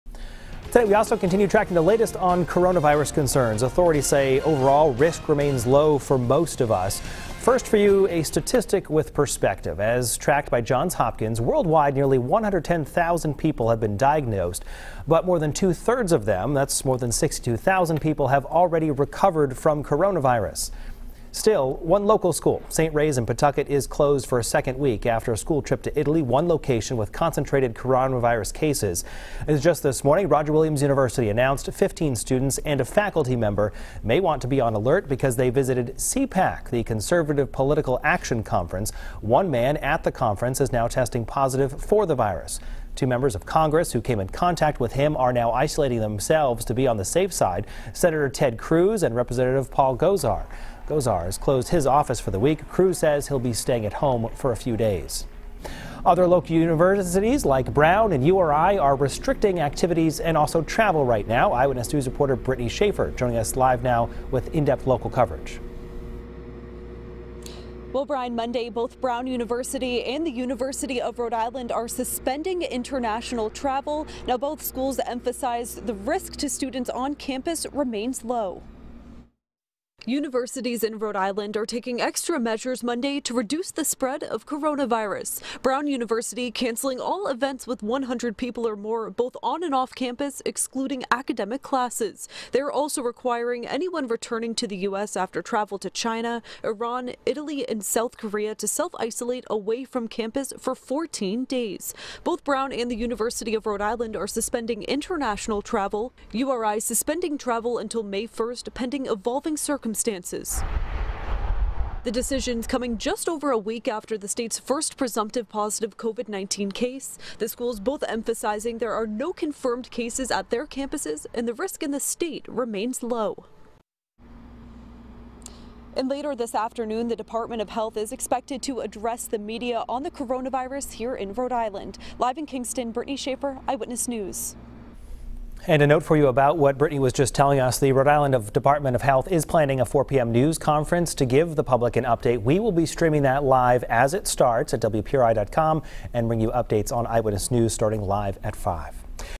Coronavirus latest
An in-depth local coronavirus update, including what extra precautions R.I. universities are taking.